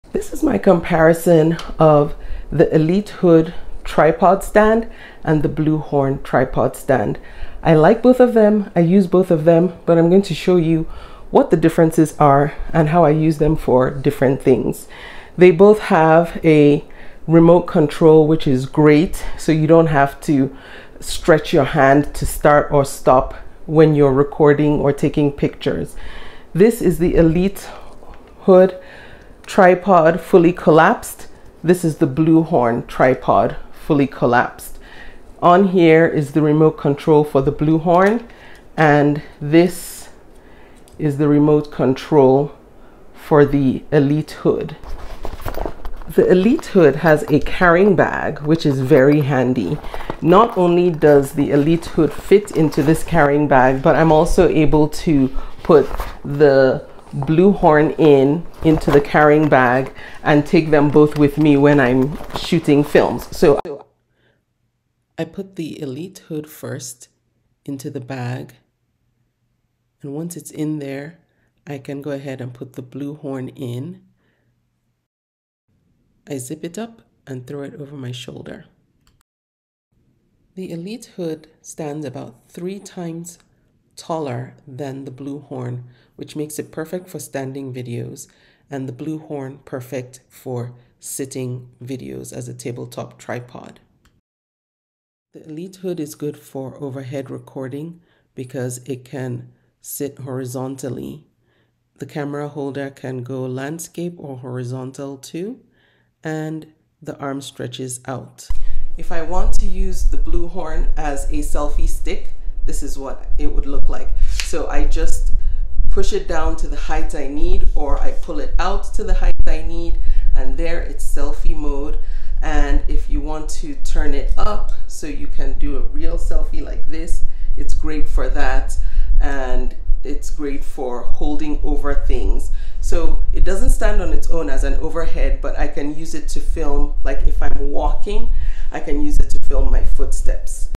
Elitehood 2023 Newest Ultra-Stable Overhead Tripod for iPhone vs Selfie Stick Portable Review
Comparison review of the Selfie Stick Portable 40 Inch Aluminum Alloy Selfie Stick Phone Tripod with Wireless Remote Shutter Compatible with All Cell Phones for Selfie/Video Recording/Photo/Live Stream/Vlog and the Elitehood 2023 Newest Ultra-Stable Overhead Tripod for iPhone – Heavy Duty Aluminum Tripod & 25in Horizontal Long Phone Arm, Tall Phone Stand Mount for Recording, 360°Adjustable iPhone Tripod Stand